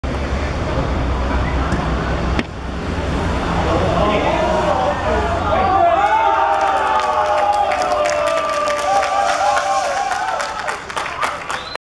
沙田vs南區(2015.1.21.香港甲組足球聯賽)之入球0:1
南區32號 奧利華